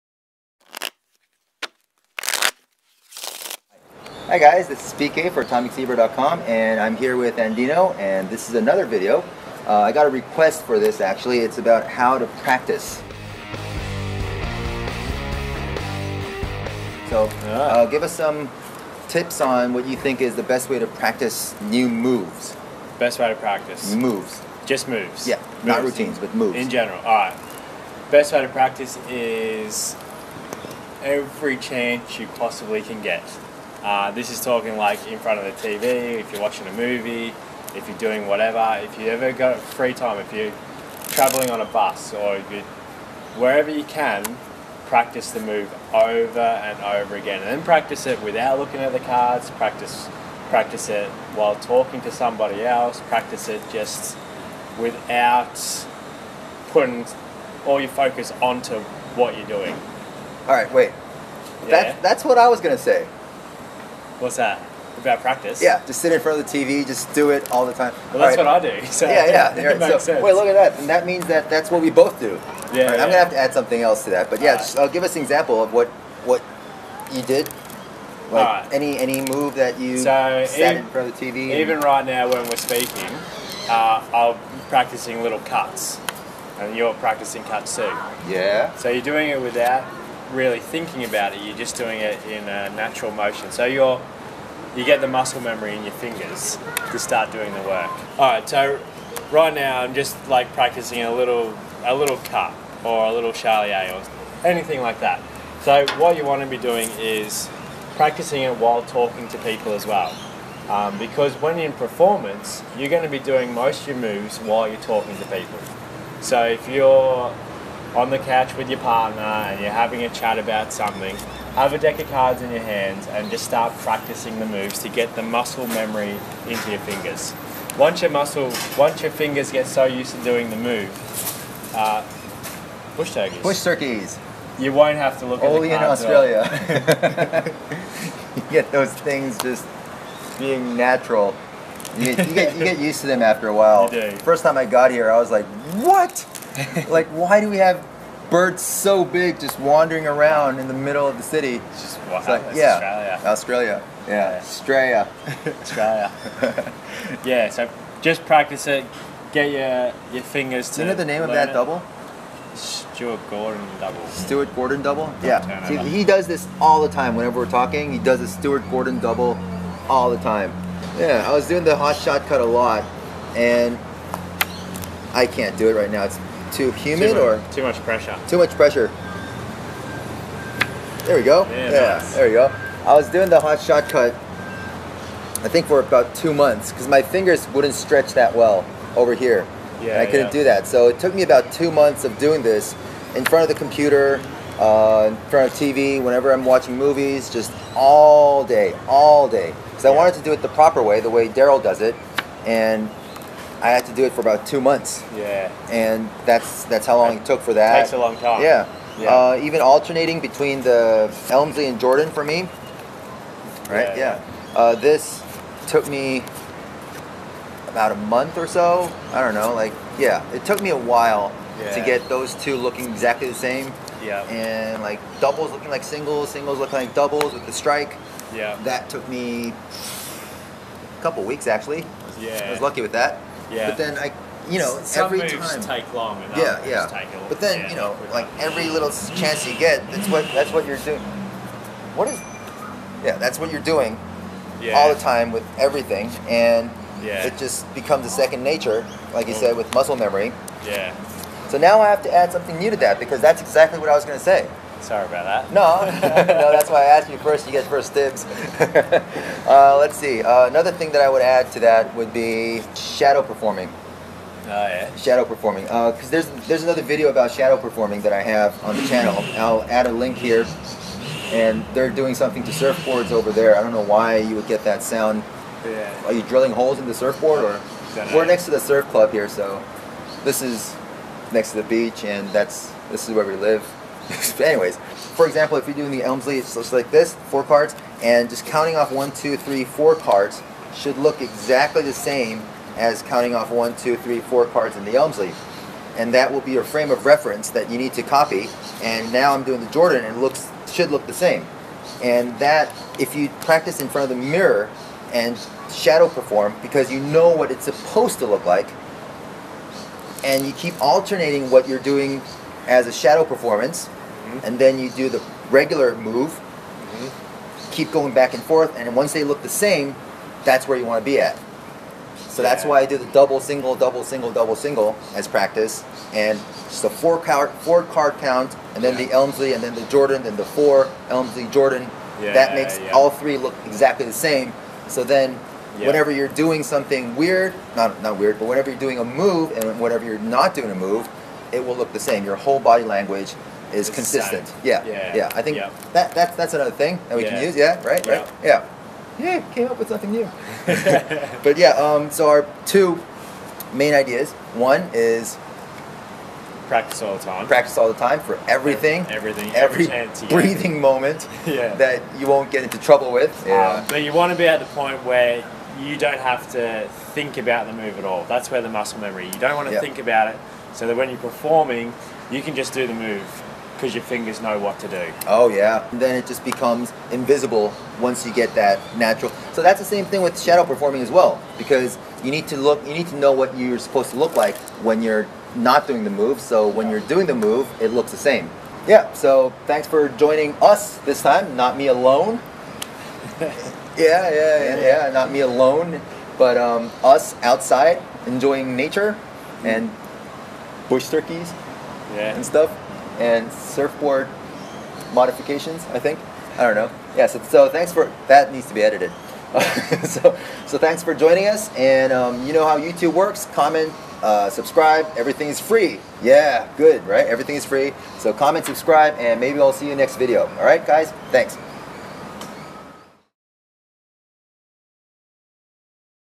AtomicZebra Magic Tips #18: How to Practice New Moves (feat. Andino)
Everyone has had to practice learning new moves. If you haven’t, you need to practice more, haha.

Andino and I discuss how to practice new moves. It’s not groundbreaking, but I’m sure we would still have some interesting everyday approaches to practicing.

This video will be part of the playlist dedicated to furthering your magic.

If you want to start learning proper sleight of hand card magic at a very affordable price, head over

New videos will be uploaded Tuesdays and Thursdays!